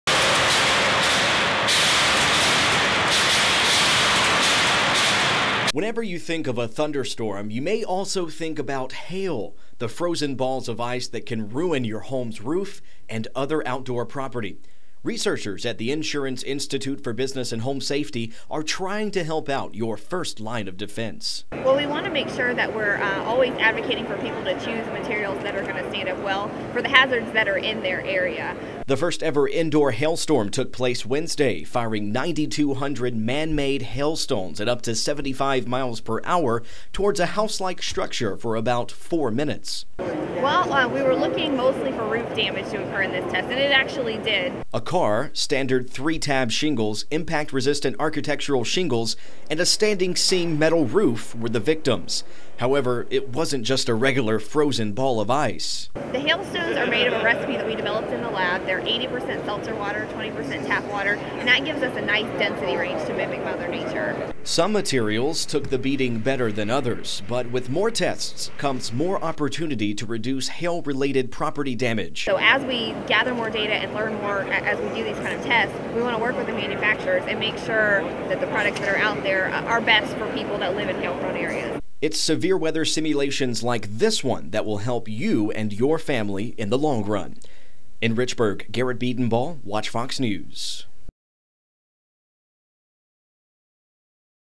First indoor hailstorm drops in SC
Man-made hailstones were shot towards a house...all in the name of research.